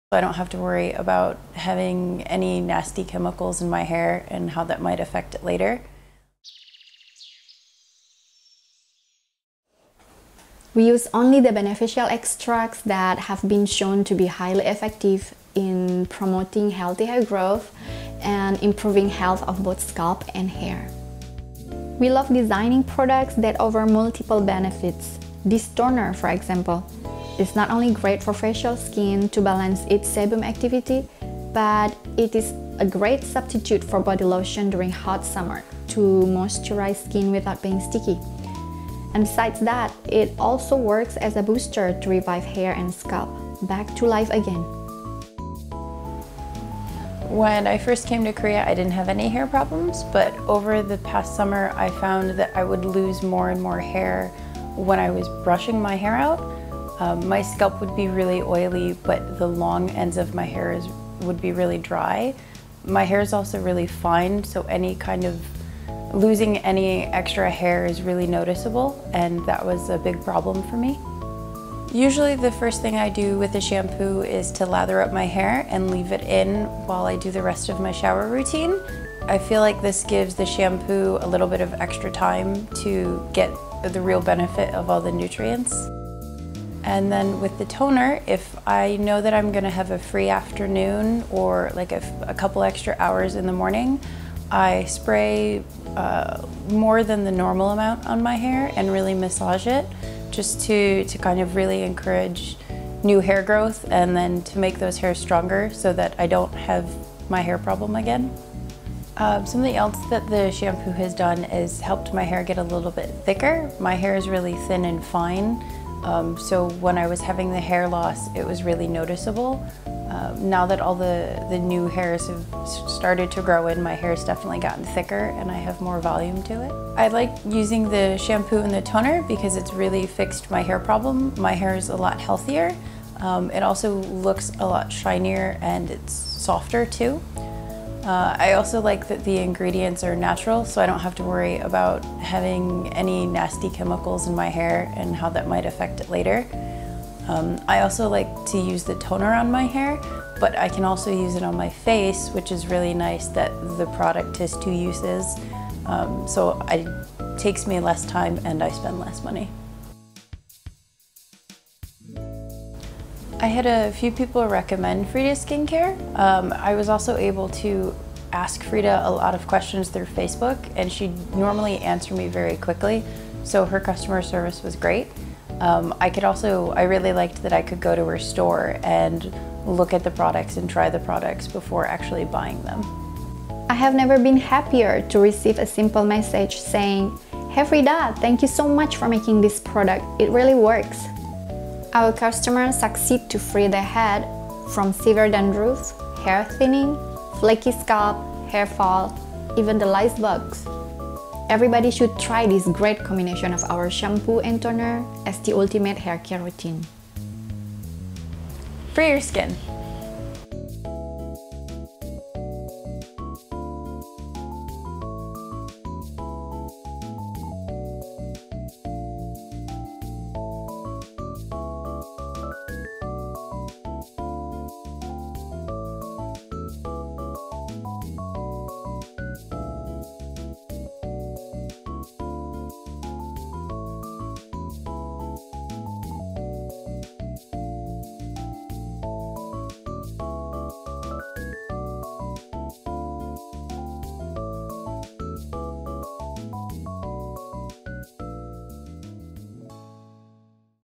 The Ultimate Haircare Routine with Natural Shampoo and Facial Toner as a Scalp Booster
A nutrient-rich shampoo to inject vitamins and moisture onto the scalp and promote hair growth. Concocted with a mixture of 5 herbal waters and 10 botanical extracts that have been shown to be highly effective in improving health of hair. Using non-comedogenic ingredients, this is a skin-friendly shampoo that does not trigger the clogging of pores during rinsing.
A combination use with our Facial Toner on the clean scalp, it will work as a booster to revive hair and scalp, back to life again. Bye bye to dandruff, flaky scalp, hair fall, and lice bugs!!

No Sodium Lauryl Sulfate (SLS). No Sodium Laureth Sulfate (SLES). No Polyethylene Glycol. No artificial colorant. No filler (paraffin, mineral oil, and any other petroleum-based oils). No alcohol. No paraben & formaldehyde preservative. No fragrance. No phthalates. No Scary stuff!
Edited by Manuela Czujewicz: ["Professional Etiquette Letter on Manners-Behavior-Character-Personality-Individuality" is available upon request]